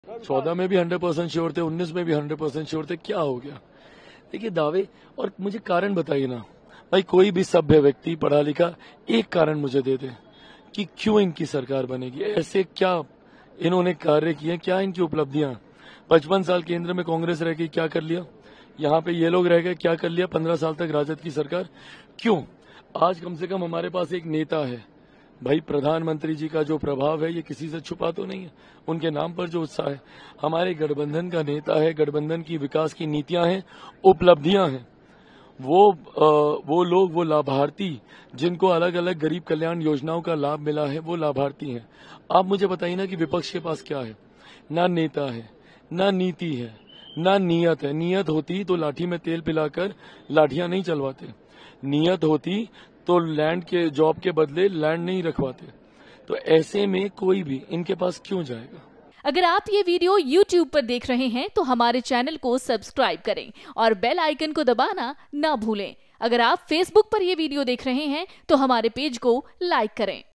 Chirag Paswan: इंडी गठबंधन पर फिर बरसे चिराग पासवान! बोले- क्यों इनकी सरकार बनेगी...| Election
पटना: LJP (रामविलास) प्रमुख और हाजीपुर लोकसभा क्षेत्र से उम्मीदवार चिराग पासवान ने कहा, "...वे(INDIA गठबंधन) मुझे एक कारण बताएं कि क्यों इनकी सरकार बनेगी? 55 साल केंद्र में कांग्रेस की सरकार रही उन्होंने क्या कर लिया?... प्रधानमंत्री मोदी ने गरीबों को योजनाओं का लाभ पहुंचाया... न उनके पास नीति है, न नीयत है और न ही नेता, फिर जनता उन्हें क्यों चुनेगी?"